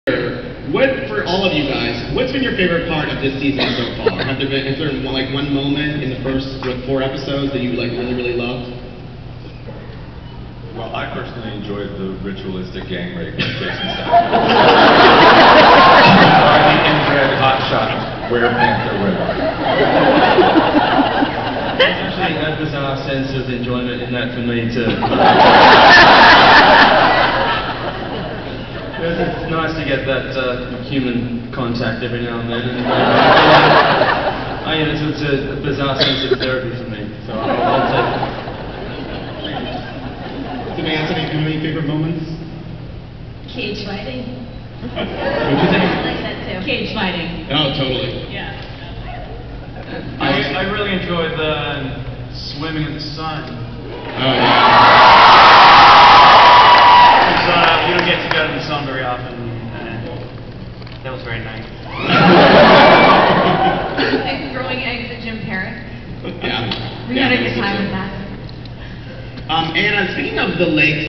Comic-Con 2011 - True Blood Panel 10
Tim asks the cast what their favorite moments of the season are so far.

All my Comic-Con videos are taken on my digital camera so please excuse the sometimes not-so-great quality.